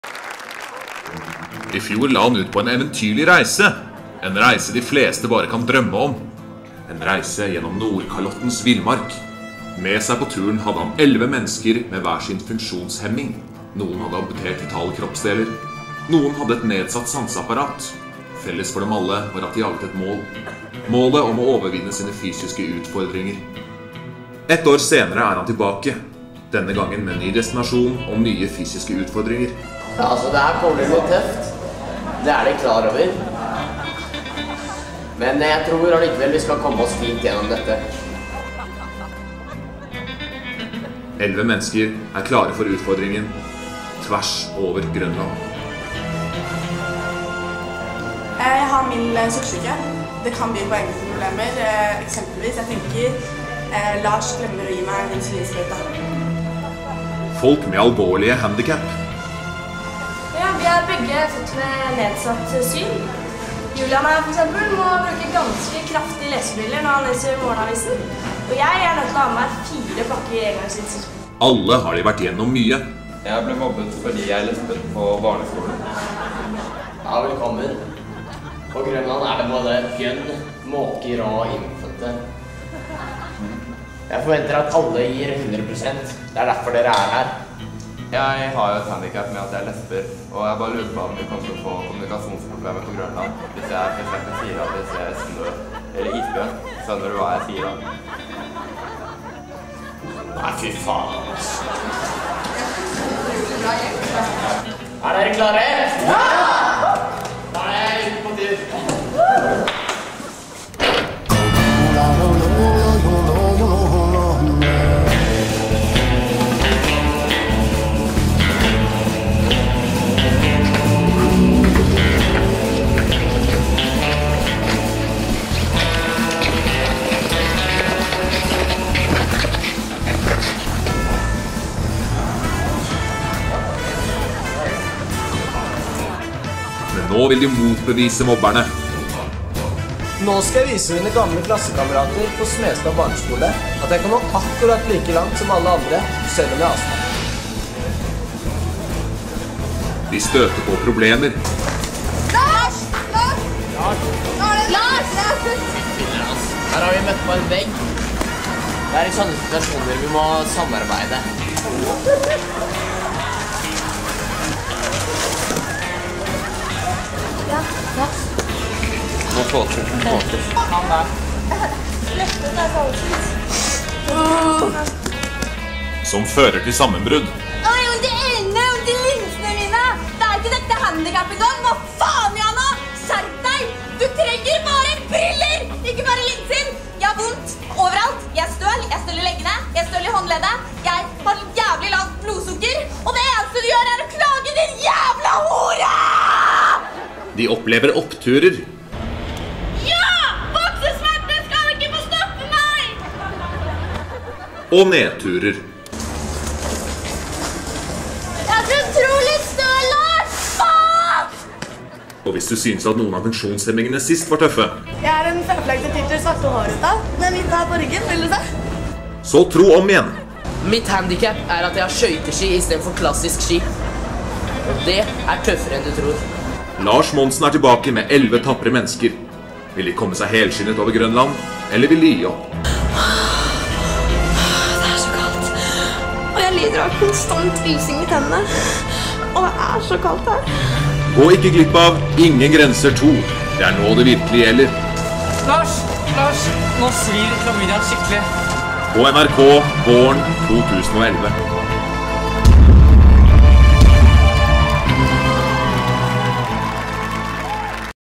OHG-Revyen 2011 ≈Grønland - Ingen grenser 2
Skuespillere: Julie Andresen, Jonas Benyoub, Gard Østby Fornes, Dina Heider Hov, Julie Mortensen Isaksen, Julian Jansson Lindseth, Patrick Krosby, Henriette Sandberg Motzfeldt, Jana Opsahl, Marcus Resch, Catharina Stoltenberg, Eivind Andreas Vold

Revysjefer: Mathias Wille, Ole Loga, Martin Helgheim, Catharina Næss

Instruktører: Stine A. Lindstad, Andreas Preus Efskin

Premiere: Tirsdag 11. januar
Karakter 6 i Aftenposten